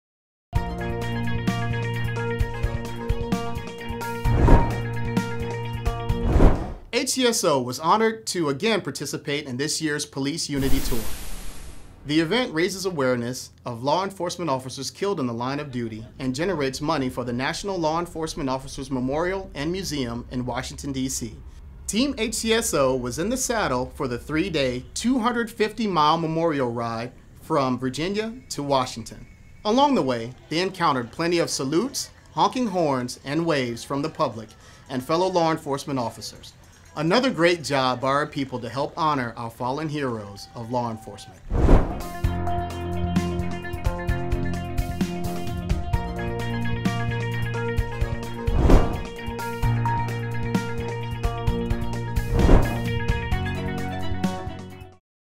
Inside HCSO Quick Hit - Police Unity Tour
HCSO was honored to again participate in this year’s Police Unity Tour. The event raises awareness of Law Enforcement Officers killed in the line of duty and generates money for the National Law Enforcement Officers Memorial and Museum in Washington, D.C.